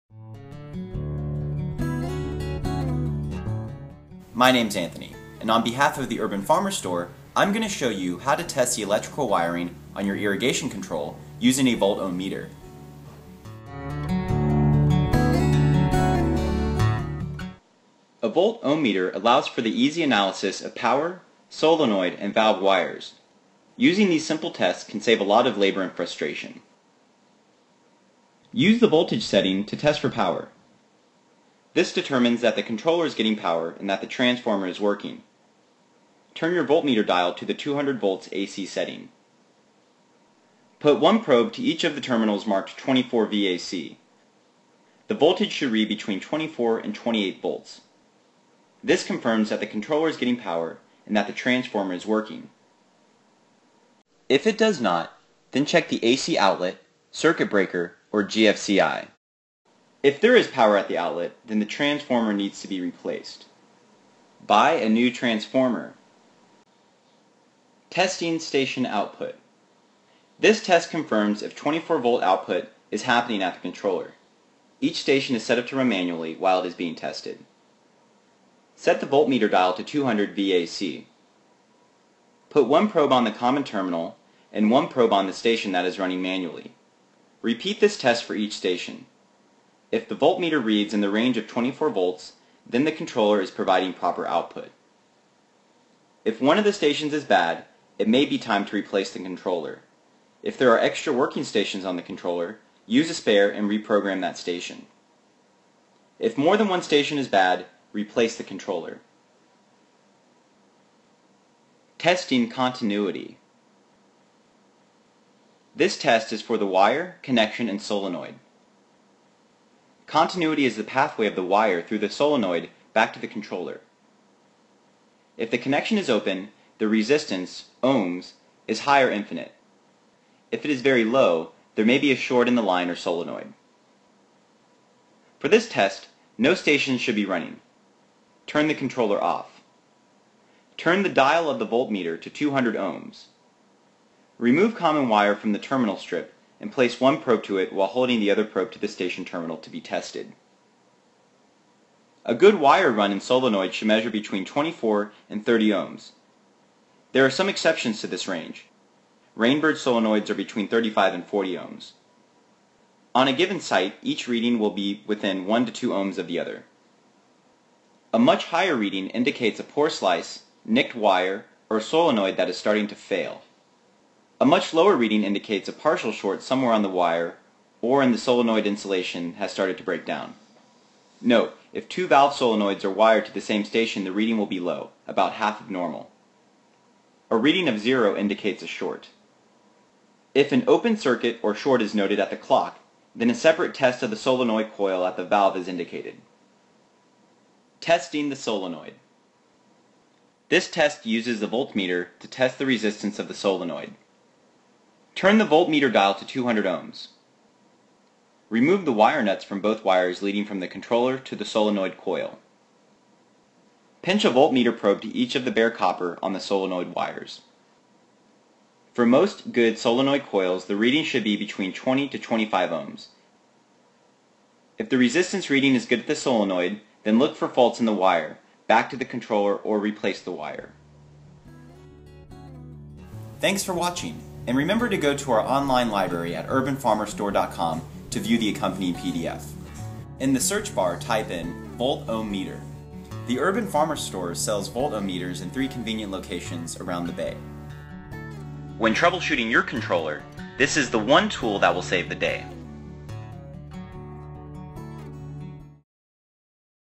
Troubleshooting Your Irrigation Control system with a Volt Ohm Meter
This video gives you step by step instructions on how to use a volt ohm meter to test the electrical wiring on irrigation controllers. The video shows how to test the station output, continuity, and solenoids.